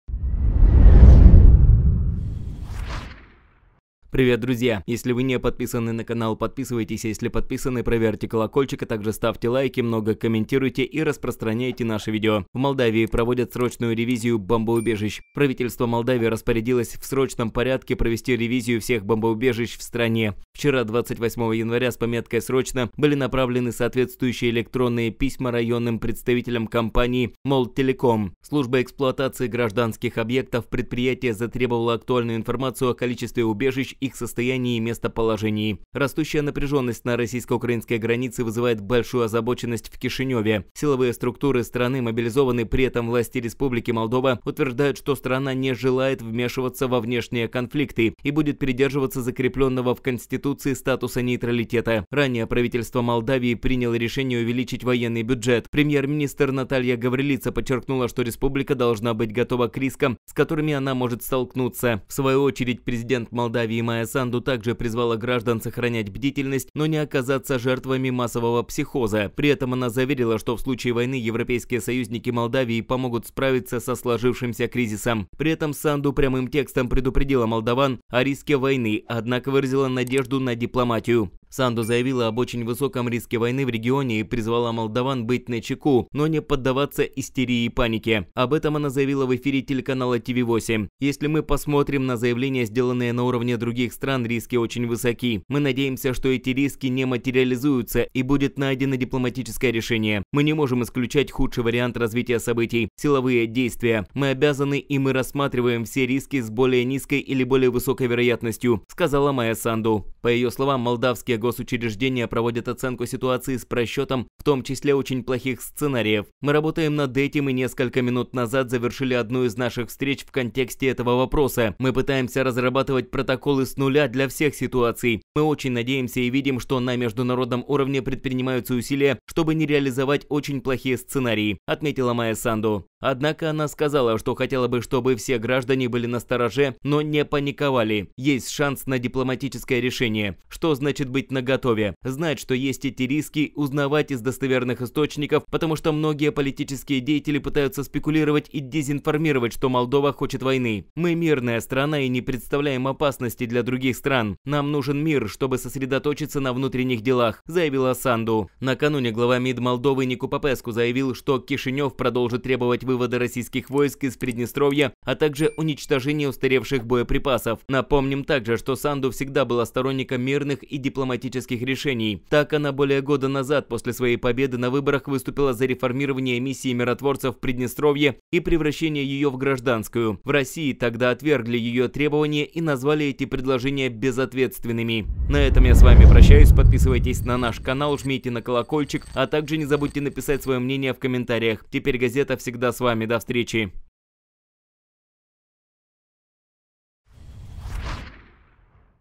Война?! Последние приготовления – срочное решение Санду: уже началось. По всей стране –что известно?
Вот о чём идёт речь:

В Молдавии проводят срочную ревизию бомбоубежищ. Правительство Молдавии распорядилось в срочном порядке провести ревизию всех бомбоубежищ в стране. Вчера, 28 января, с пометкой «срочно» были направлены соответствующие электронные письма районным представителям компании «Молдтелеком».